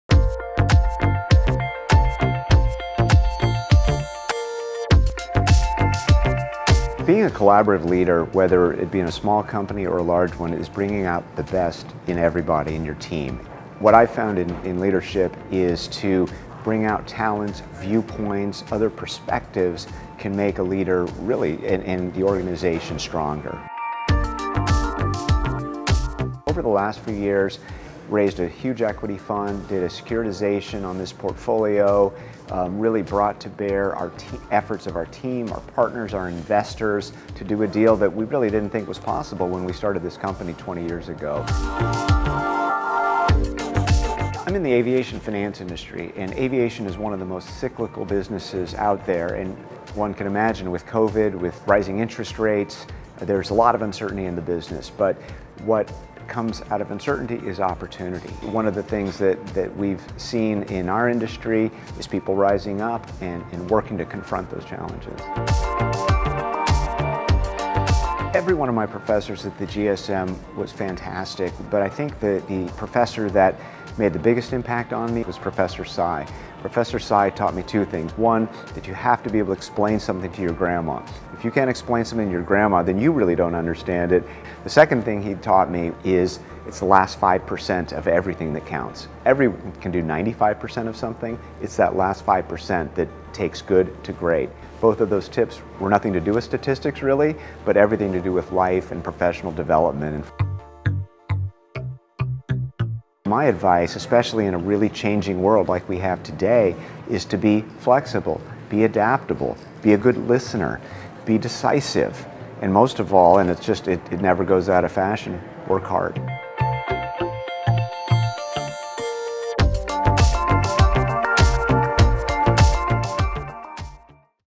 Gary Lew MBA 98 - UC Davis GSM Alumni Association - 2021-2022 Outstanding Service Award
Aviation finance industry consultant Gary Lew MBA 98 received the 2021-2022 UC Davis Alumni Association Outstanding Service Award. Each year the Graduate School of Management (GSM) Alumni Association recognizes three GSM graduates who embrace the School’s core values of passionate and energetic leadership, and who demonstrate a commitment to team building and service.